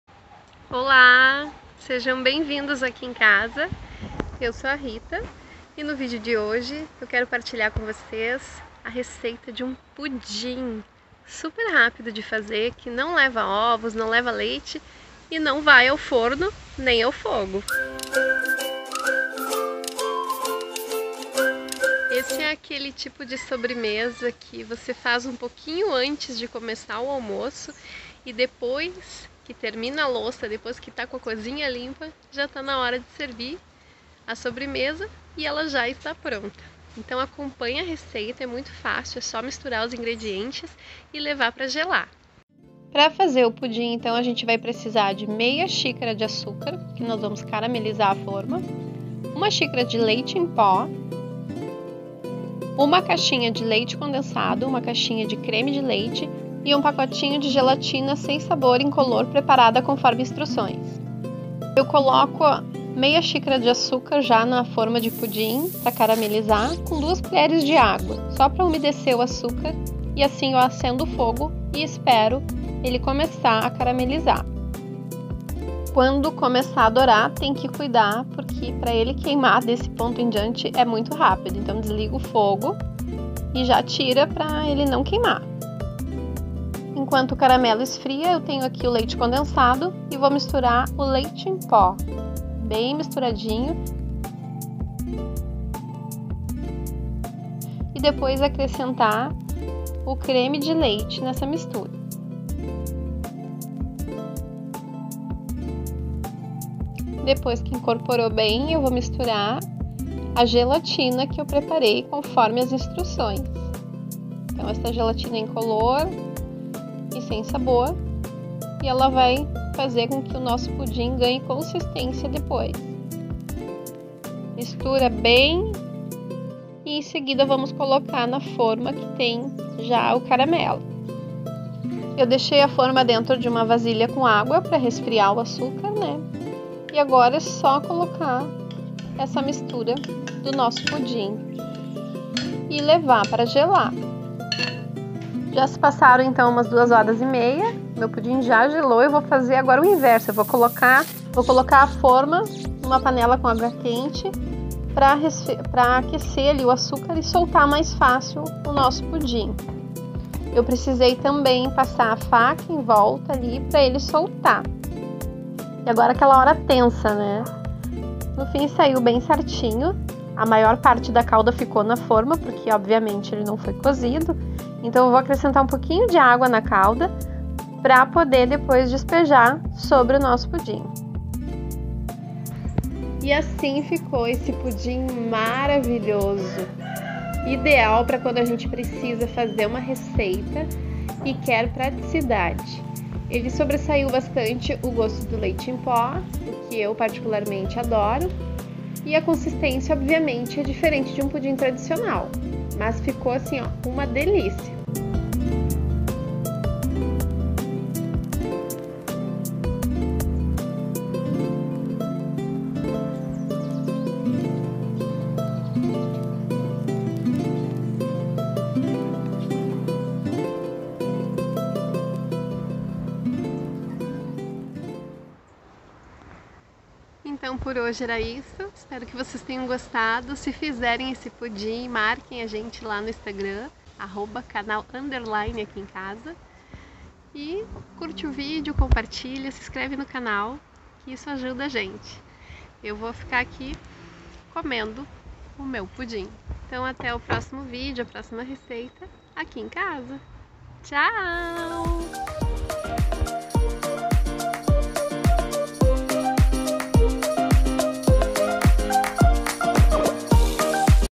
Pudim fácil e rápido que não vai ao forno
Neste vídeo partilhamos uma receita de pudim delicioso, fácil e rápido.

INGREDIENTES:

- 1/2 xícara de açúcar  + 2 col. de sopa de água para caramelizar a forma
- 1 xícara de leite em pó
- 1 caixinha de leite condensado
- 1 caixinha de creme de leite
- 1 pacote de gelatina sem sabor preparada conforme instruções da embalagem

Acompanhe nosso canal no Instagram @canal_aquiemcasa